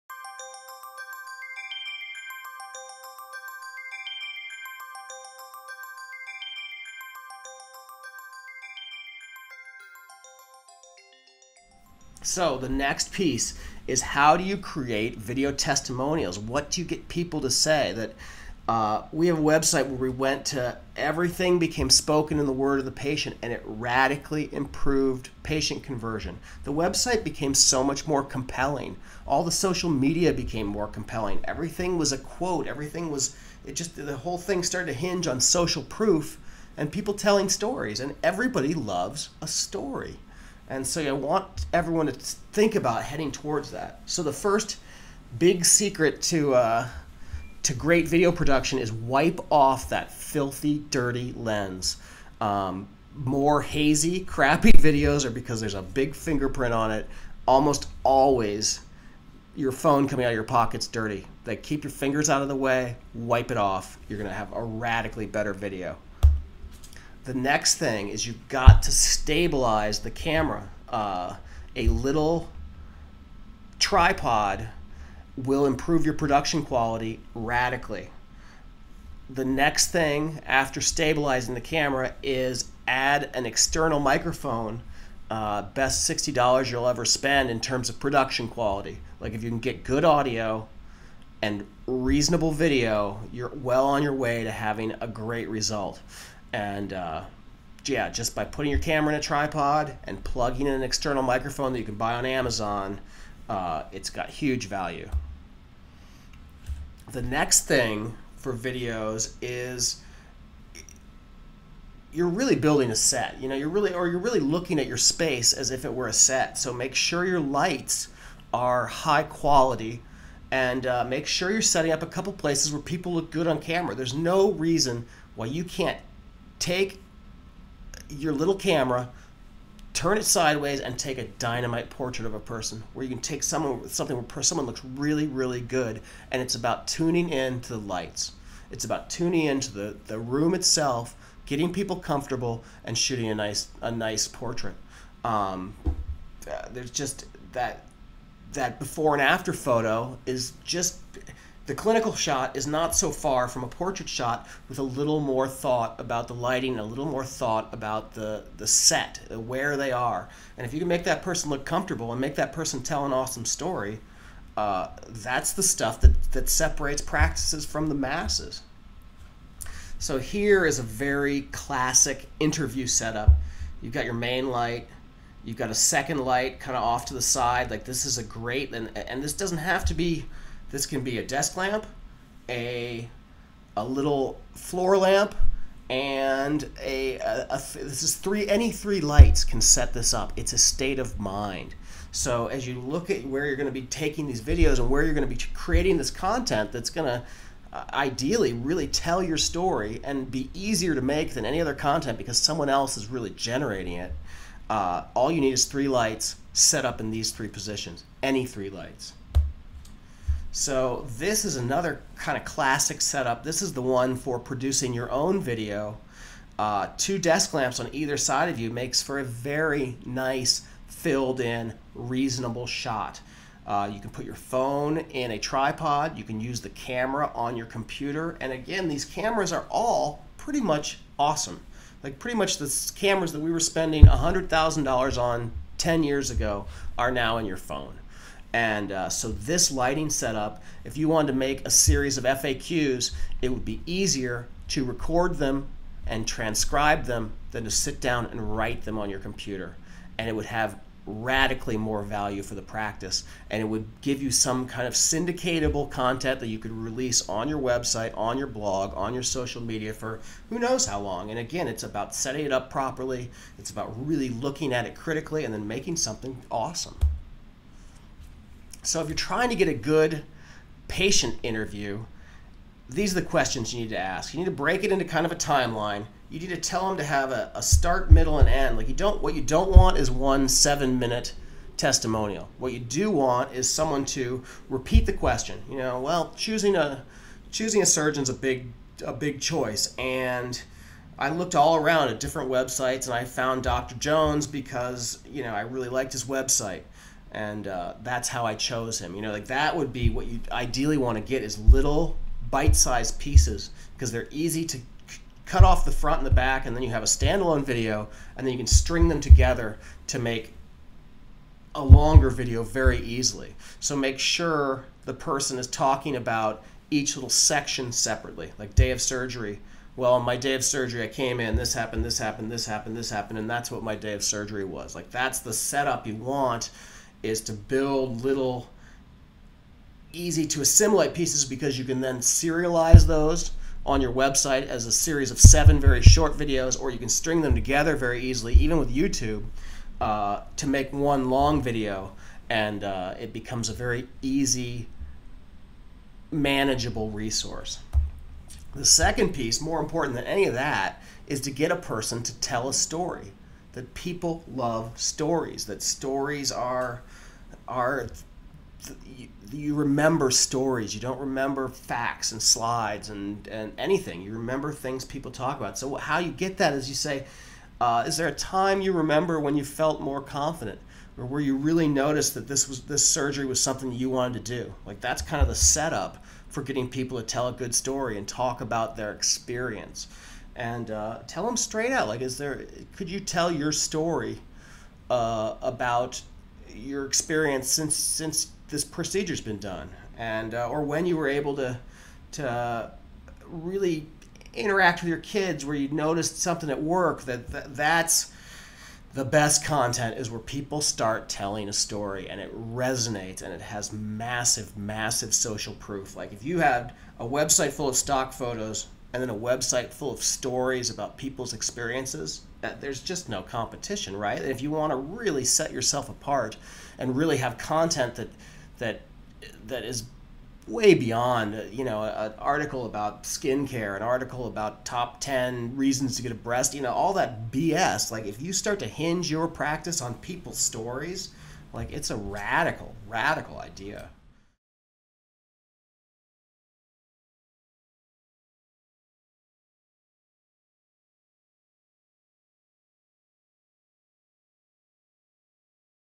Producing Patient Stories and Video For Marketing
Comprehensive guide to shooting patient photo and video for clinical and marketing use.